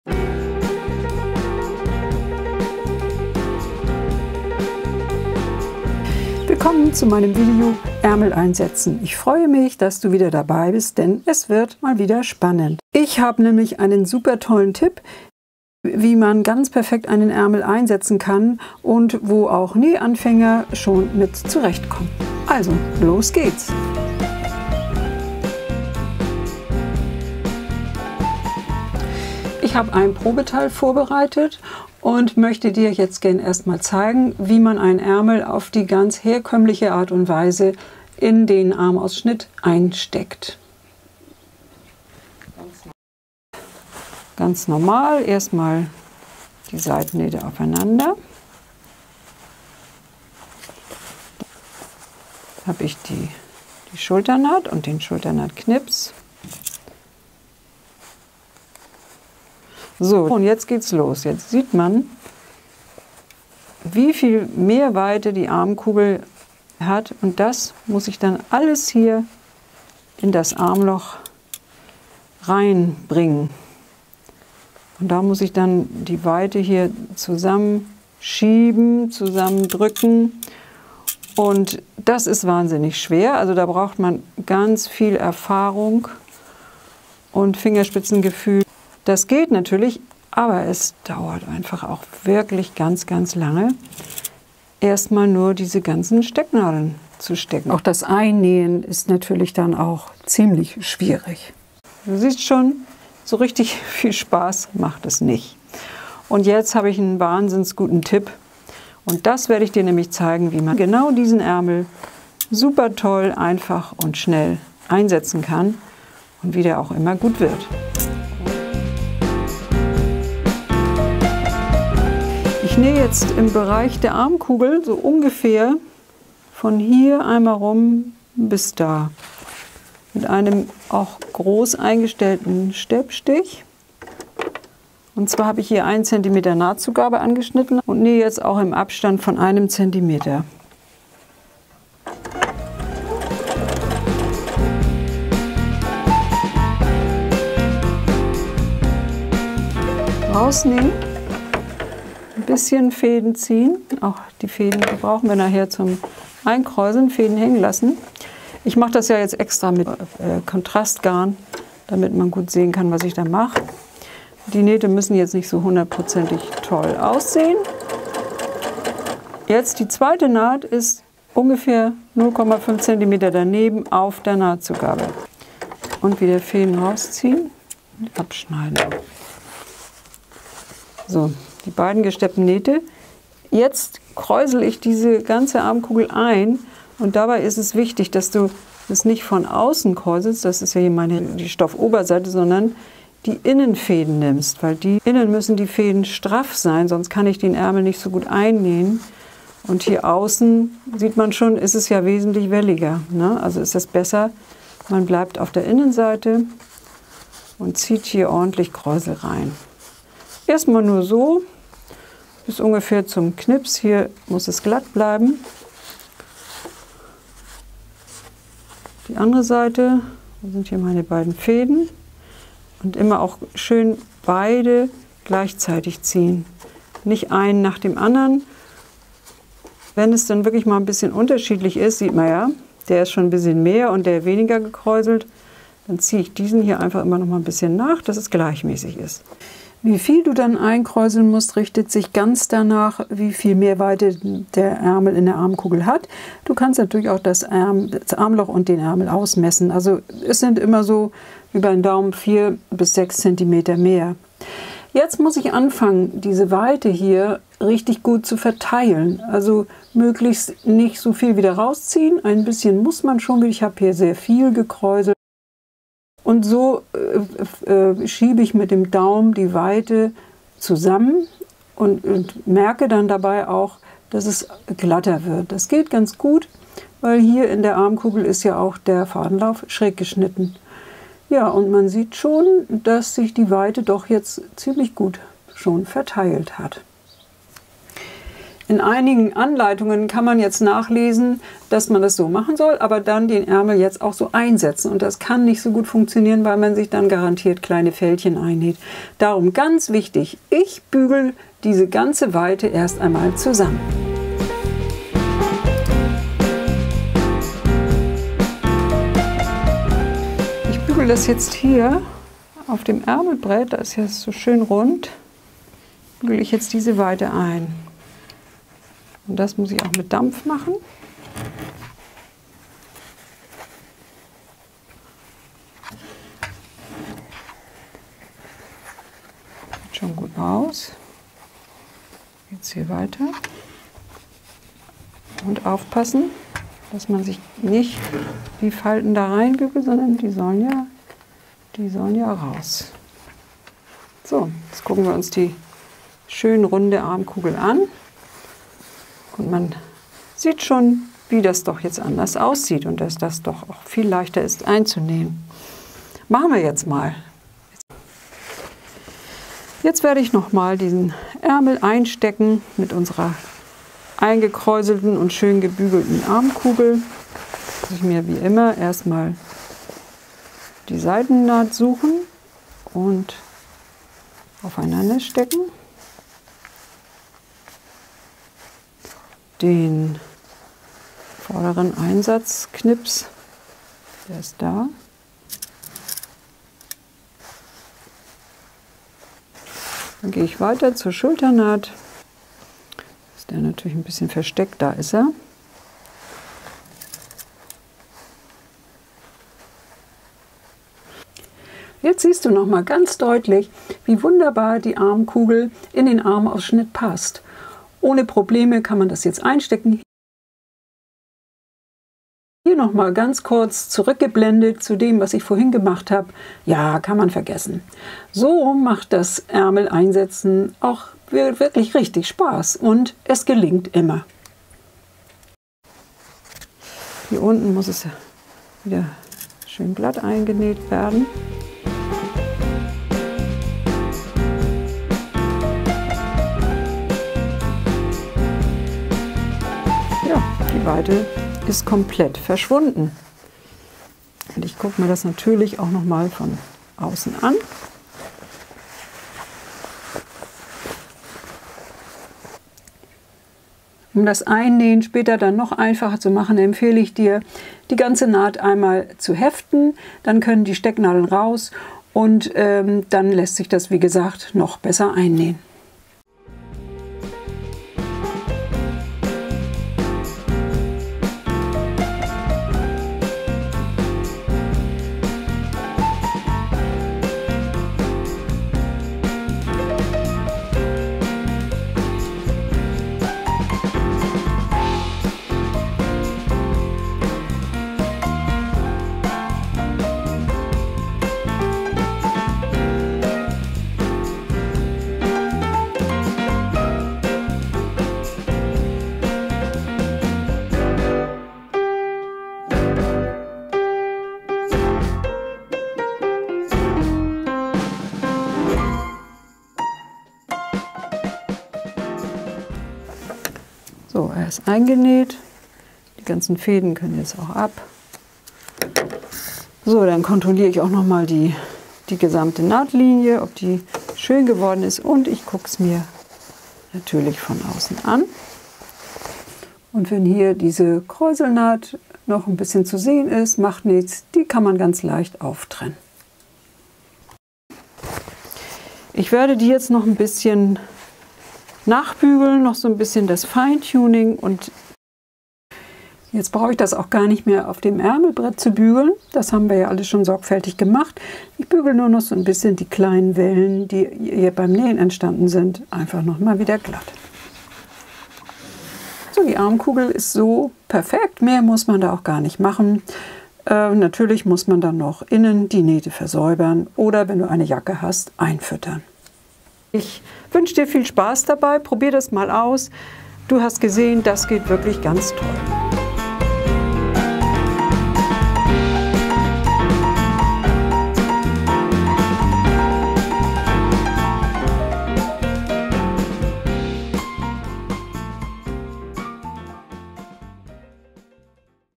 Ärmel einsetzen ohne Stress – So einfach geht’s!
In diesem Video zeige ich dir, wie einfach es ist, einen Ärmel perfekt in einen Armausschnitt einzusetzen! Zuerst bereiten wir die Armkugel zum Einkräuseln vor. Danach kräuseln wir die Armkugel und bügeln sie glatt. Der letzte Schritt – das Einsetzen des Ärmels – ist dann schnell und unkompliziert. Lass uns gemeinsam diese Technik meistern!